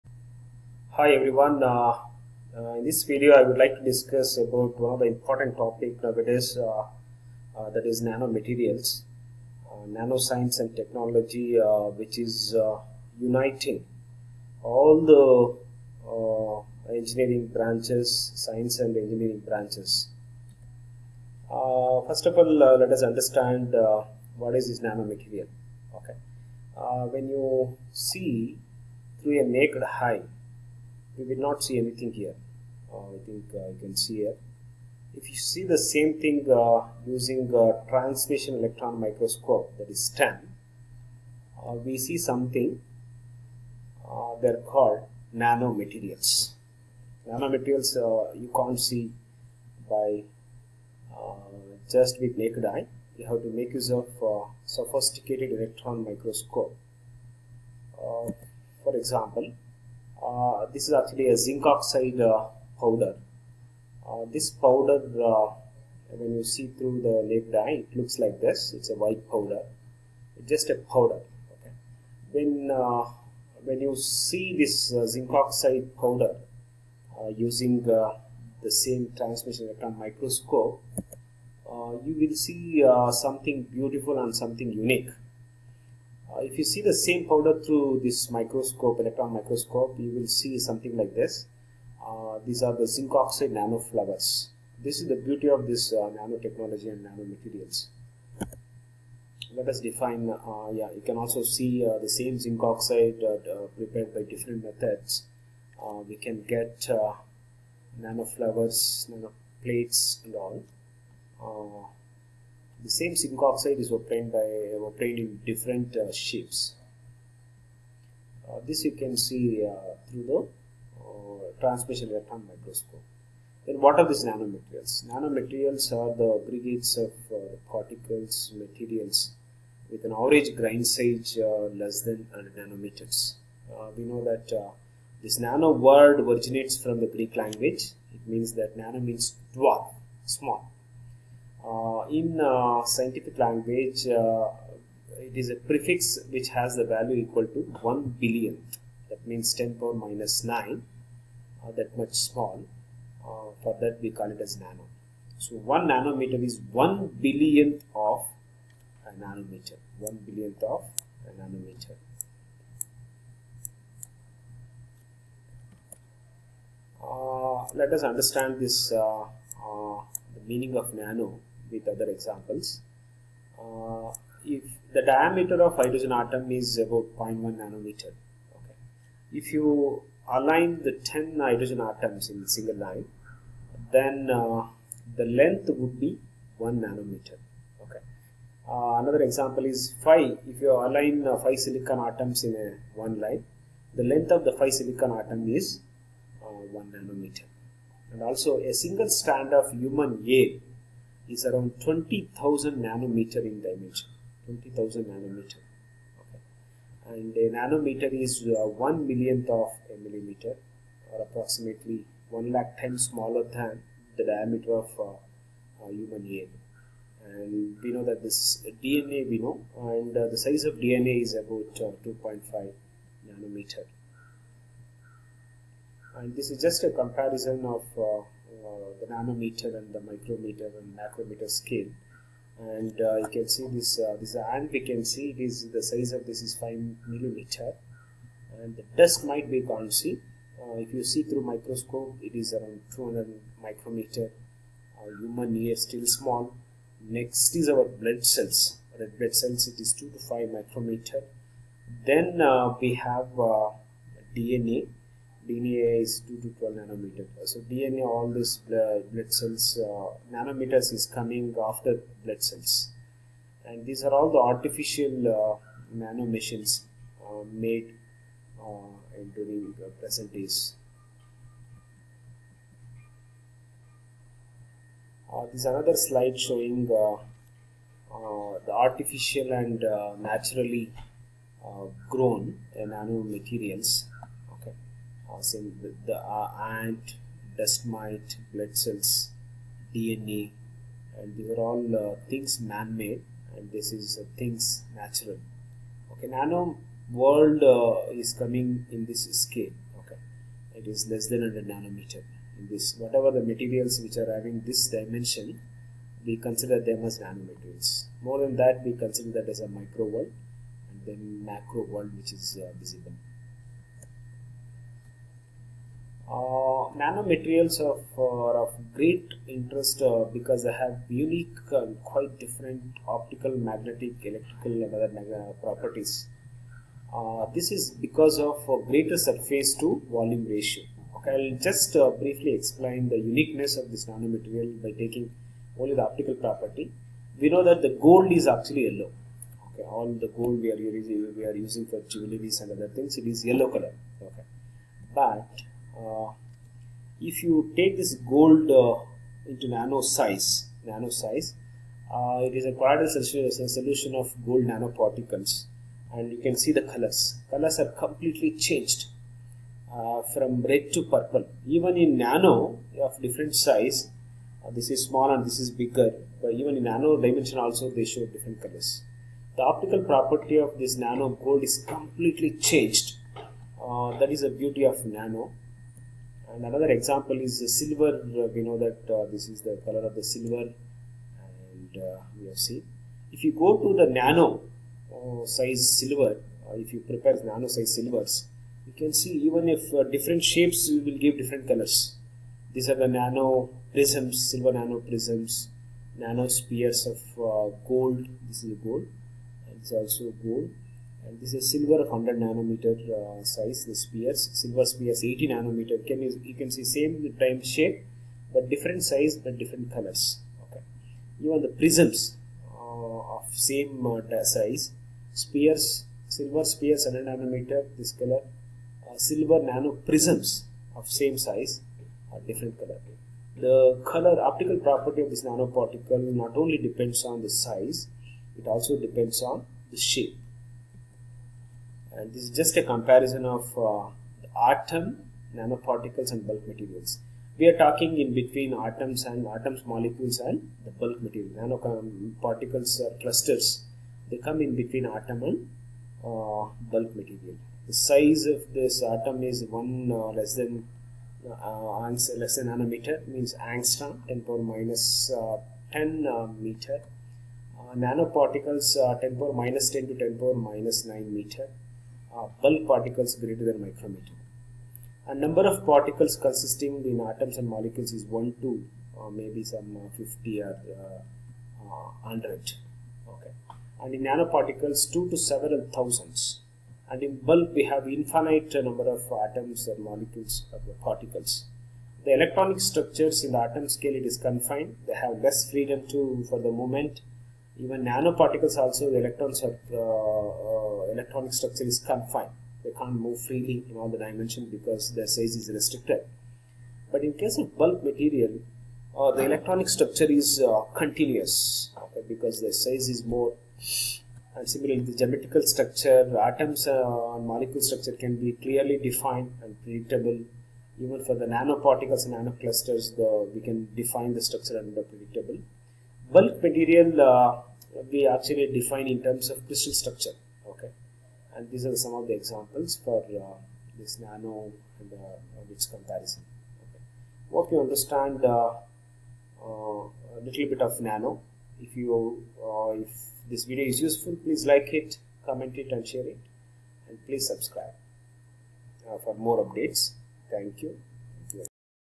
Introduction to Nano dimensional materials
in this video, i have discussed about the nanomaterials by comparing with atoms , molecules and bulk materials with suitable examples. Uniqueness of nanomaterials is also discussed by comparing the optical property of bulk and nano gold and silver as an example.